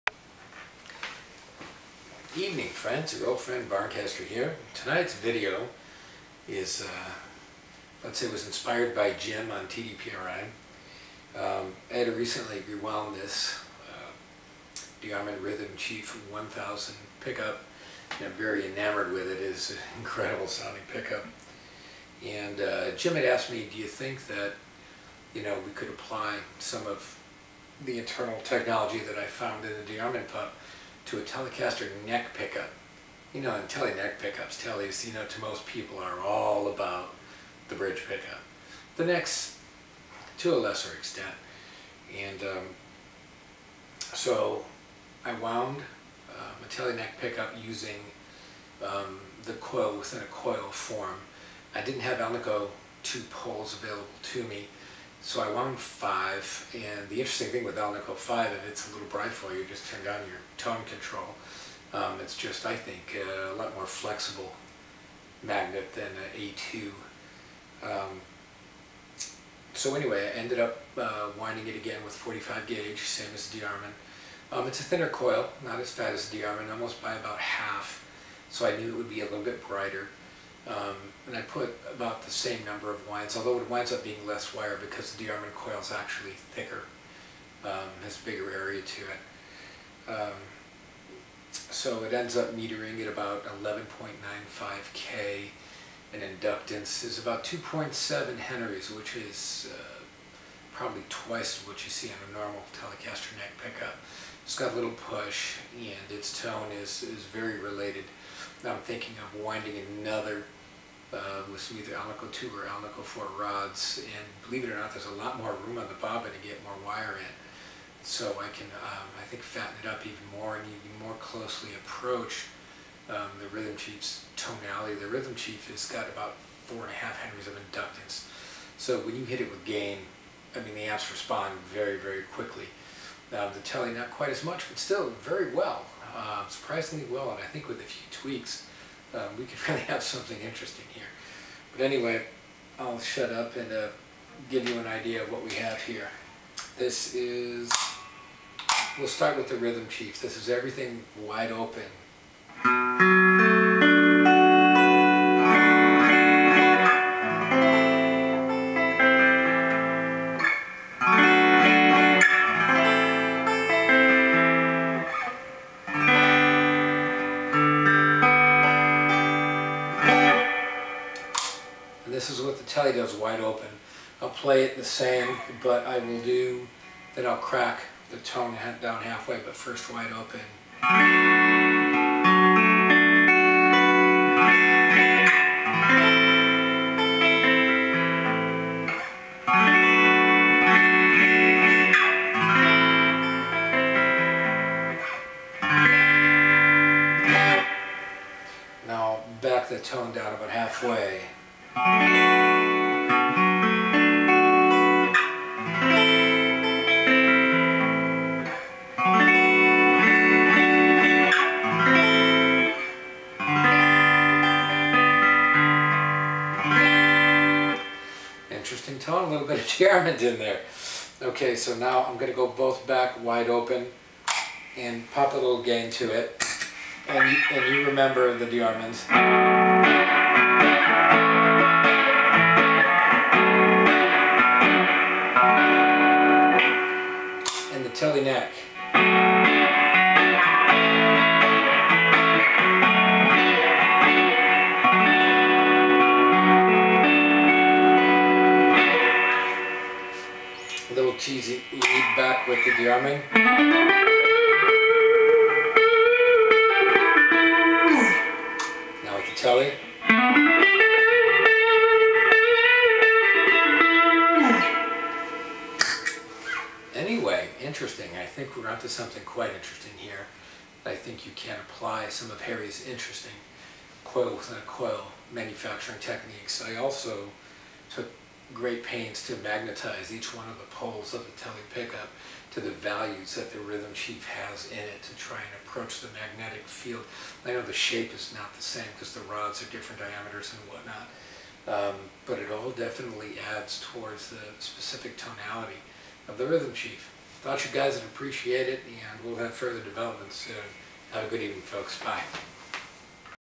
Tele Neck Pup with a Little Rhythm Chief 1000 Added
For all my friends at TDPRI. Here is a Tele neck pup that was wound in the style of a DeArmond Rhythm Chief 1000. 45 AWG wire was used. The pole pieces were magnetized to the gauss levels found in a Rhythm Chief 1000. The tone is coming along nicely. The next version will have a few more tweaks to close in on the RC.